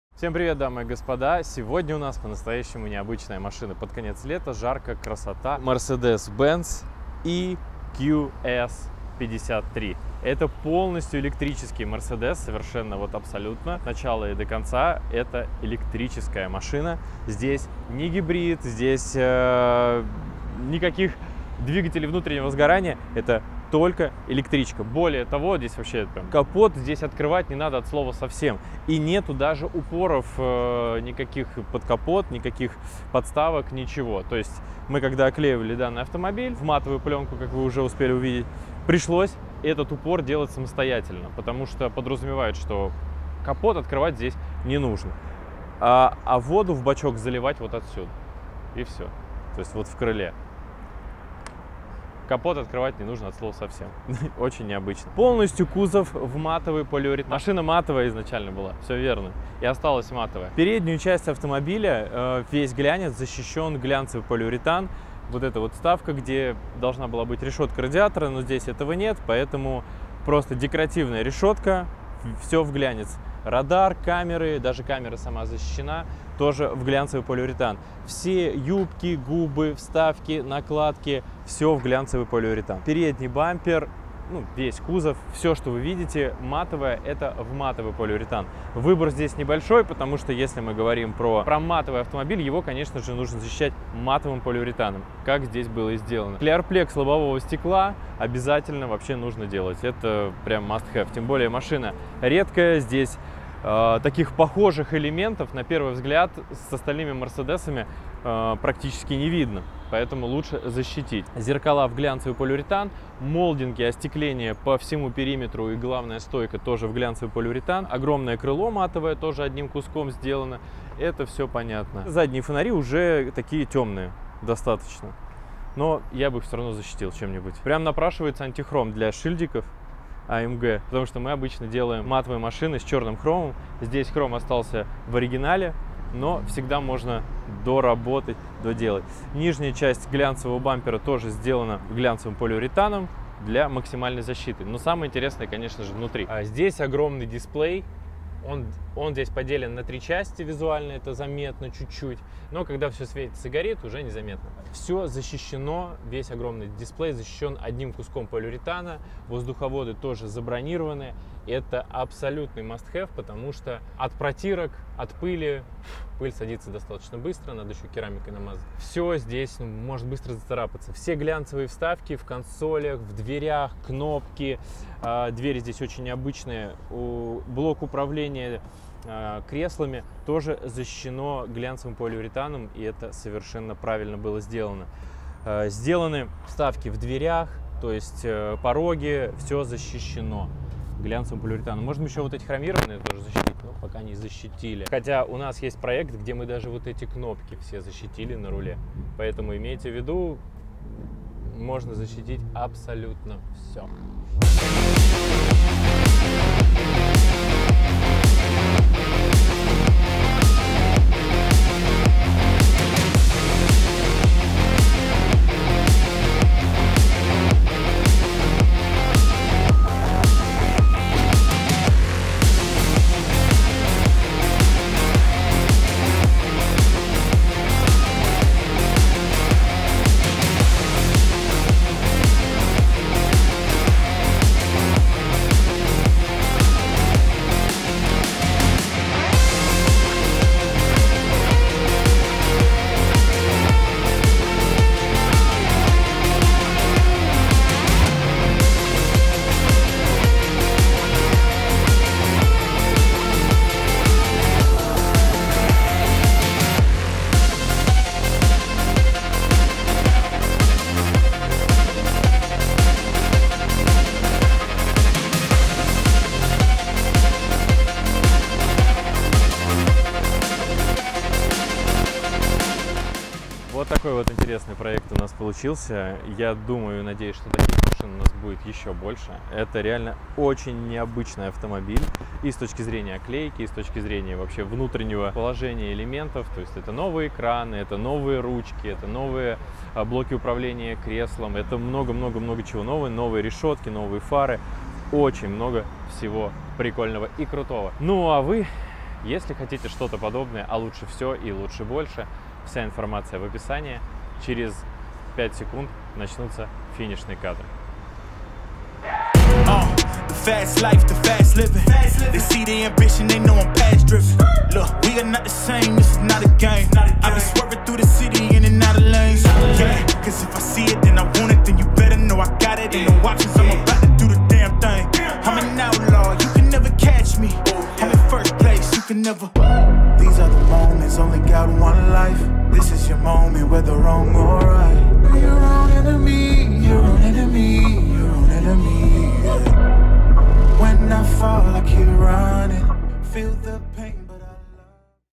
Новый электрокар от Mercedes | Матовый Mercedes-AMG EQS 53 под матовой пленкой
Лето не переставало удивлять нас электрокарами. Вслед за электронными Фольксвагенами к нам приехал матовый Mercedes-AMG EQS 53. А что нужно делать с матовым Мерседесом? Конечно же защищать матовой пленкой и не только...

Выполненные работы:
1️⃣ Детейлинг мойка
2️⃣ Бронирование лобового стекла ClearPlex
3️⃣ Оклейка авто матовой пленкой
4️⃣ Оклейка консоли и глянцевых элементов салона пленкой
5️⃣ Бронирование фар

Адрес по ссылке откроется в навигаторе:
5-ый Донской проезд 19